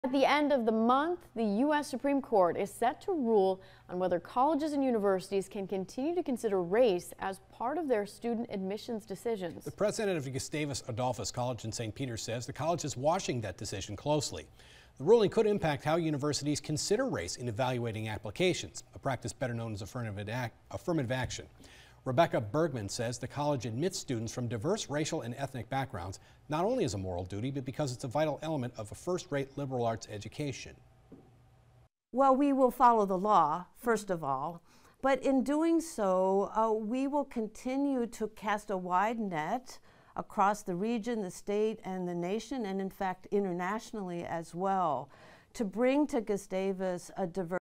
U.S. Supreme Court to decide whether colleges can consider race in admissions process
The President of Gustavus Adolphus College in St. Peter says the college is watching the decision closely.